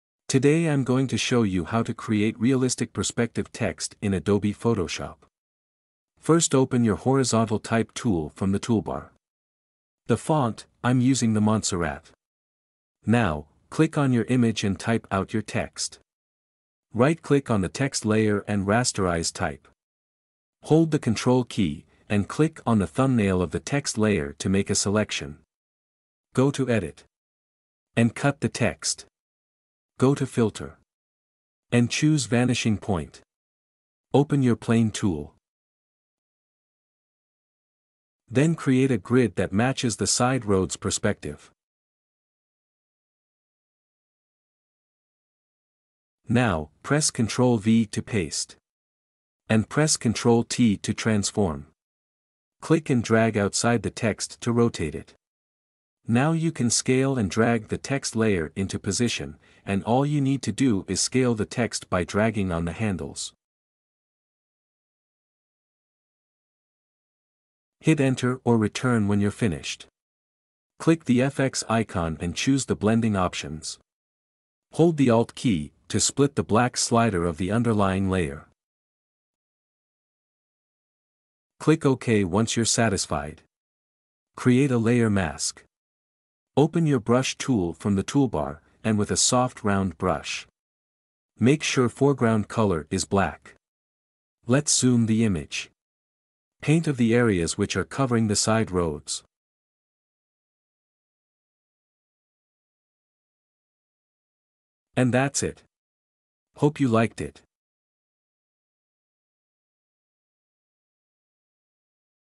How to Create Realistic Perspective Text - Photoshop Tutorial
In this video, I'm going to show you how to Create Realistic Perspective Text in Adobe Photoshop